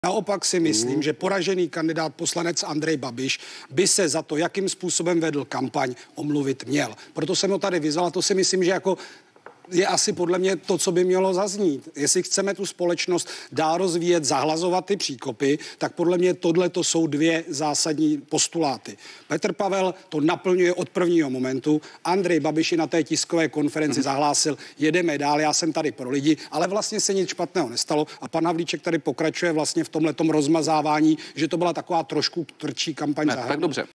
Andrej Babiš by se měl omluvit za svou kampaň!
Andreji Babiši, měl byste se omluvit za to, jak jste vedl kampaň. Část vašich voličů se bojí totality či války, kterou jste hrozil. Zachovejte se prosím jednou v životě správně a omluvte se. Uklidněte své voliče. Pokud chceme zahlazovat příkopy, musí na tom pracovat obě strany.